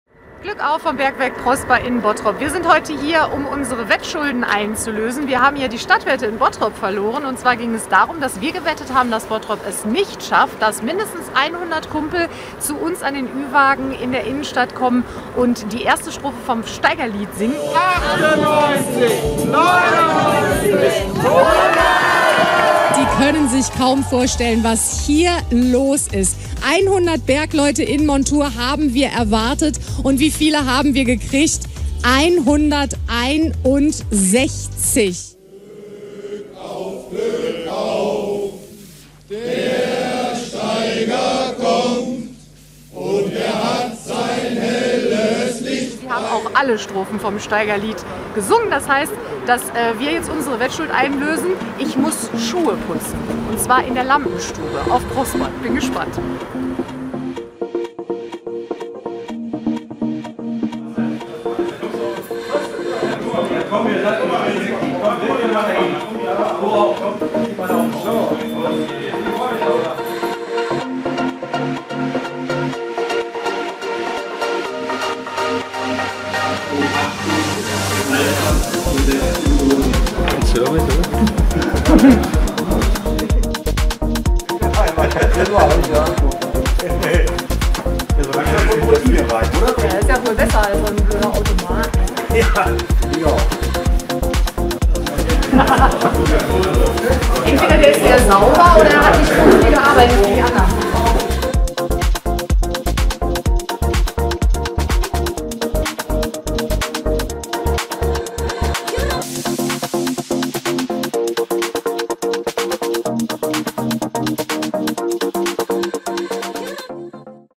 Wettschulden - Corinna Schröder putzt Schuhe auf Prosper
Schuhe putzen auf Prosper! Corinna Schröder hat ihren verlorenen Wetteinsatz eingelöst. Die Kumpels haben jetzt sauberere Schuhe denn je... ;-)